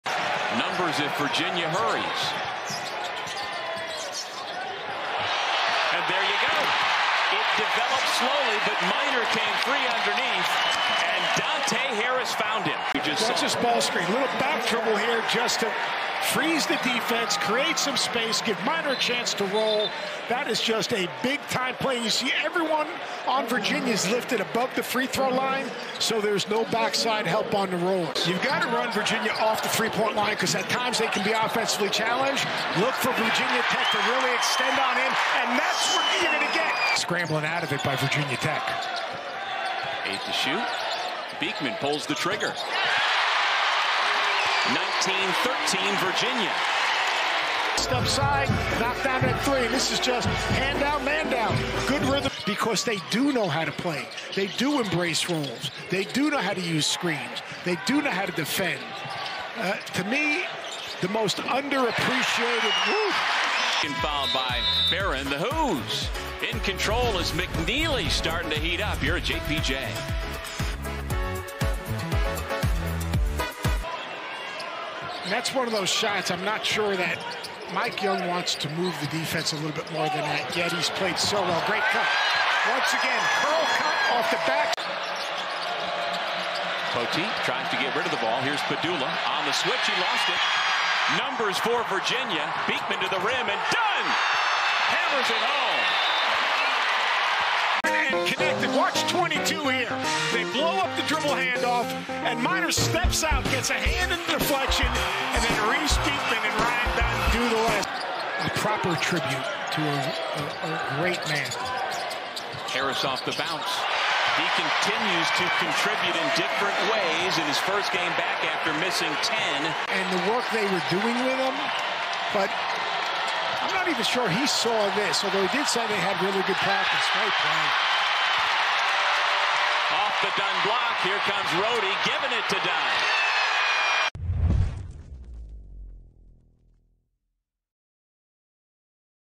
Virginia Cavaliers Highlights vs. Virginia Tech Hokies
Broadcast highlights from tonight's win vs. Virginia Tech!